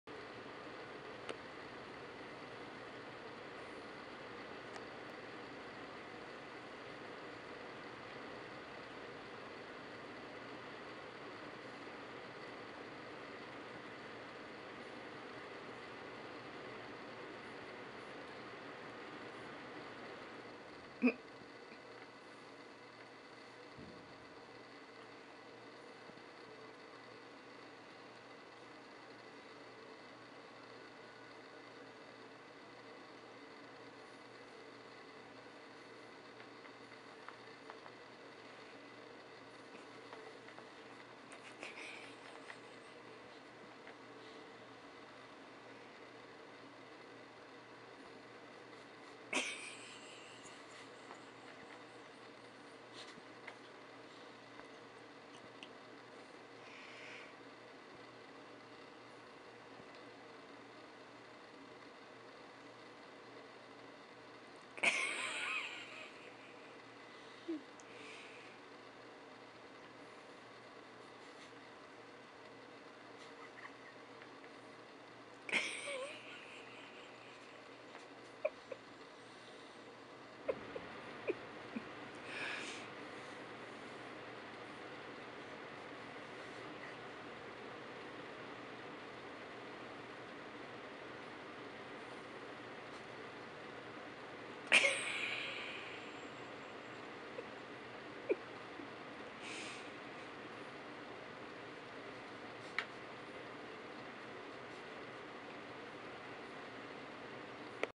Mac cleaning foot
Cleanliness is next to bunnliness!
Who know a bunny's foot requires so much cleaning.  :o)